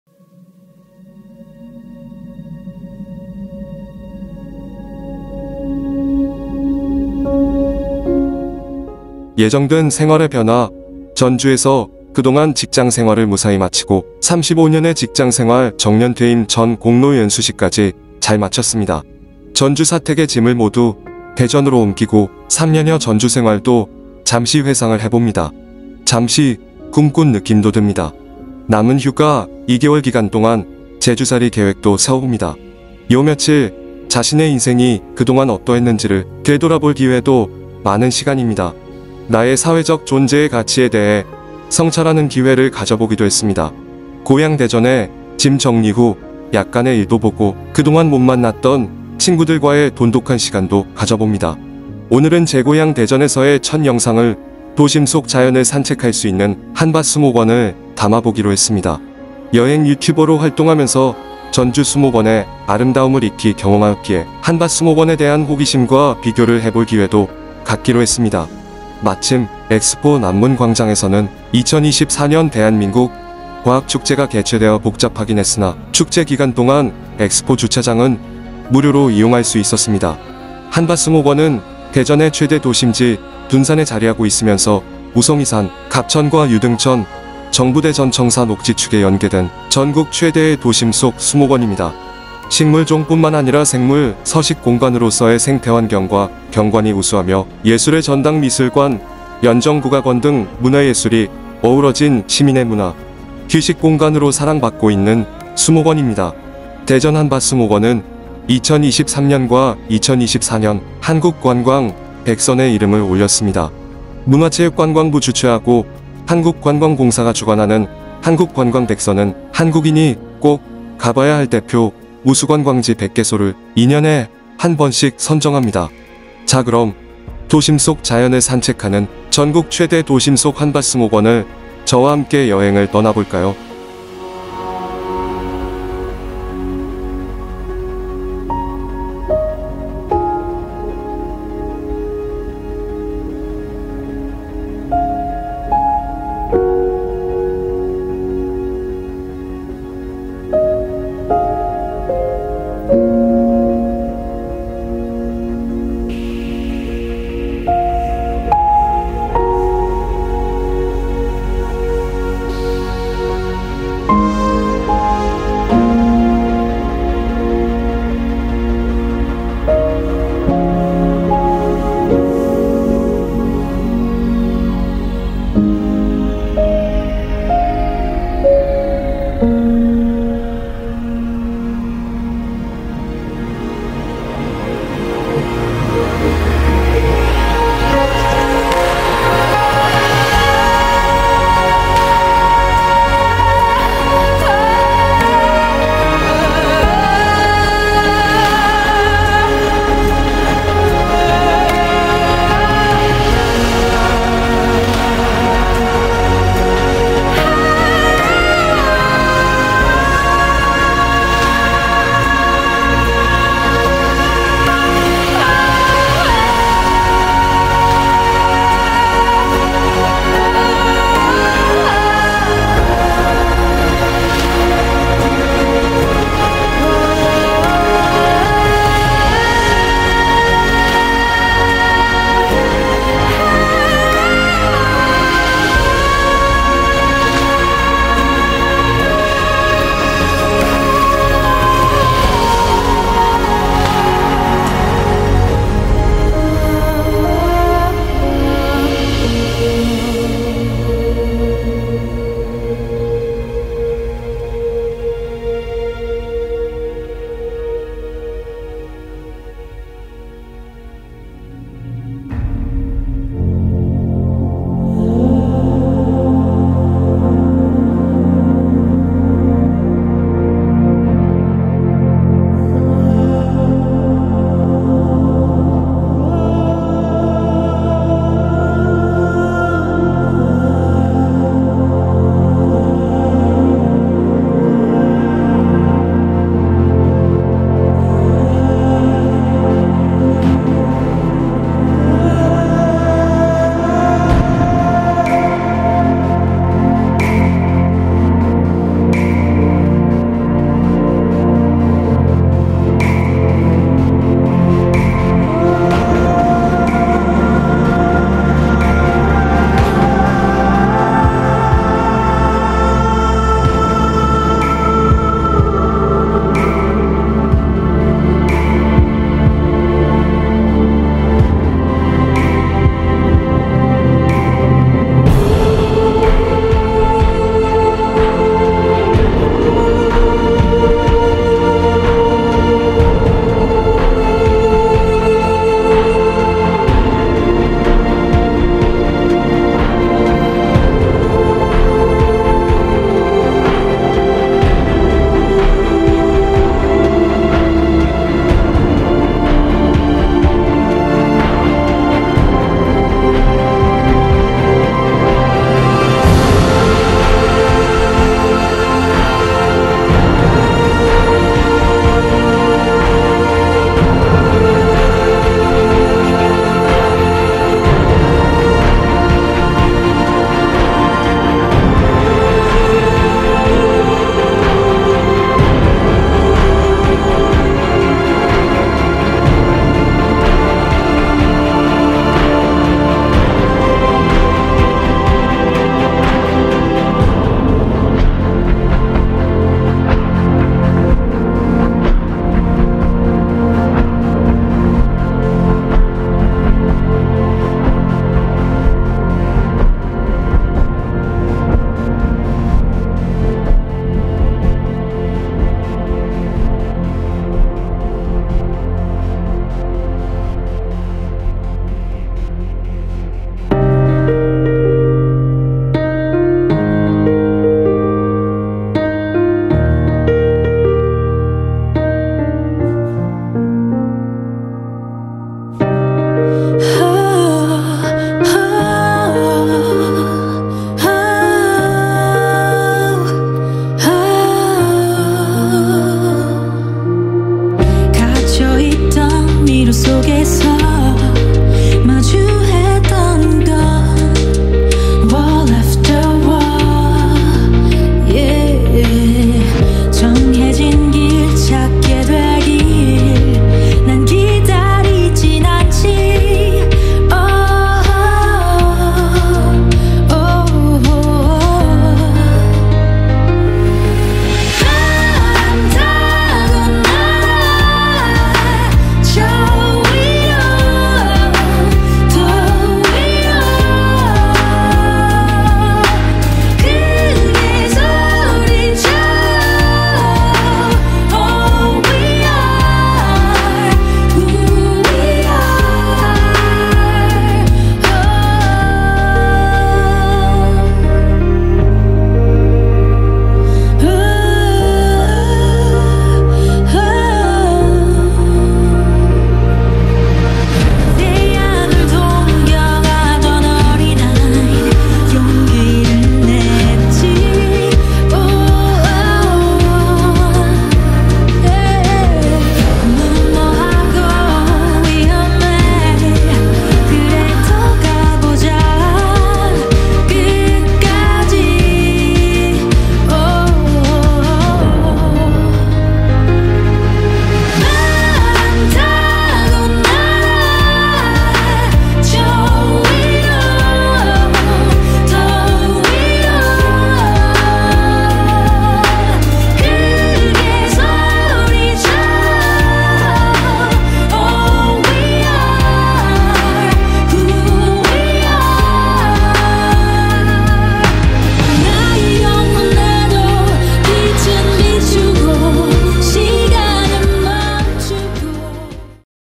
"도심 속 자연을 산책하는" 전국 최대 도심속 수목원 '도심 속 허파', 2023~2024 한국관광 100선 선정 대전의 명소 대전 한밭수목원, 대전에서 제일 먼저 가볼 곳
예정된 생활의 변화, 전주에서 그동안 직장생활을 무사히 마치고,
35년의 직장생활 정년퇴임 전 ‘공로연수식' 까지 잘 마쳤습니다.
전주 사택에 짐을 모두 대전으로 옮기고 3년여 전주생활도 잠시
회상을 해봅니다.  잠시 꿈꾼 느낌도 듭니다.  남은 휴가 2개월
기간동안 ‘제주살이' 계획도 세워봅니다.  요 며칠, 자신의 인생이
그동안 어떠했는지를 되돌아볼 기회도 많은 시간입니다.
나의 사회적 존재의 가치에 대해 성찰하는 기회를 가져보기도
했습니다.  고향 대전에 짐 정리후, 약간의 일도 보고, 그동안
못 만났던 친구들과의 돈독한 시간도 가져봅니다.

오늘은 제고향 대전에서의 첫 영상을 ‘도심속 자연을 산책할수 있는
한밭수목원을 담아보기로 했습니다.  여행유튜버로 활동하면서
전주수목원의 아름다움을 익히 경험하였기에 '한밭수목원'에 대한
호기심과 비교를 해볼 기회도 갖기로 했습니다.  마침, 엑스포 남문
광장에서는 2024년 대한민국 과학축제가 개최되어 복잡하긴 했으나,
축제 기간동안 엑스포 주차장은 무료로 이용할수 있었습니다.
한밭수목원은 대전의 최대 도심지 둔산에 자리하고 있으면서 우성이산,
갑천과 유등천, 정부대전청사 녹지 축에 연계된 전국 최대의 도심 속
수목원입니다.  식물 종(種) 뿐만 아니라 생물 서식공간으로서의
생태환경과 경관이 우수하며, 예술의 전당·미술관·연정국악원 등
문화예술이 어우러진 시민의 문화·  휴식공간으로 사랑받고 있는
수목원 입니다. 대전 한밭수목원은 ‘2023년과 2024년 한국관광
100선'에 이름을 올렸습니다.  문화체육관광부 주최하고 한국관광공사가
주관하는 한국관광 100선은 한국인이 꼭 가봐야 할 대표 우수관광지
100개소를 2년에 한번씩 선정합니다.  자 그럼, 도심 속 자연을
산책하는 전국 최대 도심속 한밭수목원을 저와 함께 여행을 떠나볼까요

[대전 한밭수목원 가는방법 관람시간 이용요금 주차요금]
ㅇ 주소 대전광역시 서구 둔산대로 169
ㅇ 이용시간 [4월~10월(하절기)]
          - 동원 05:00~21:00
          - 서원 05:00~21:00
          - 열대식물원 09:00~18:00
         [11월~3월(동절기)]
         - 동원 07:00~19:00

24년 4월 화창한 도심 속 자연을 산책하는 대전의 명소 한밭수목원은
전국 최대 도심속 수목원 '도심 속 허파' 대전 한밭수목원이라 불리기도 합니다.
대전시에서 명품화 구상 시동을 걸었던 대전 한밭수목원, 자연 안에서 쉬어 가세요!